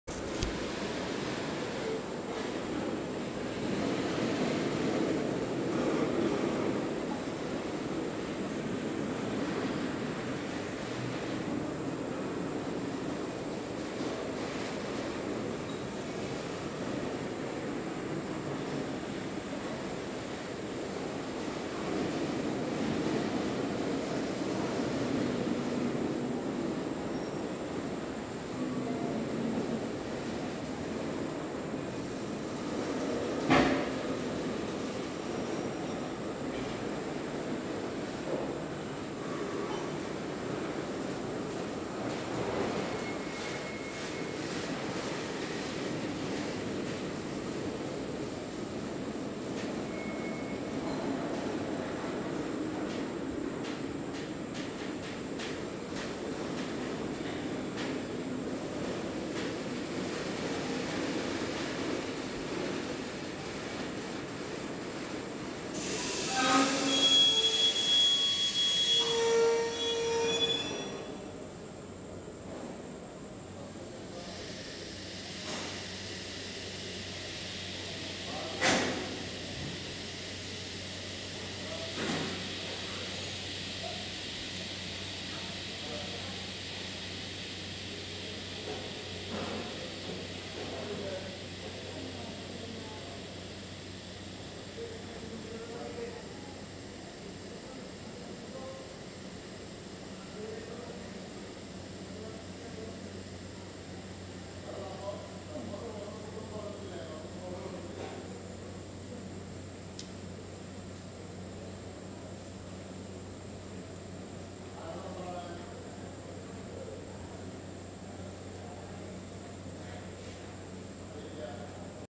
goods wagons
The slow stopping of a goods train.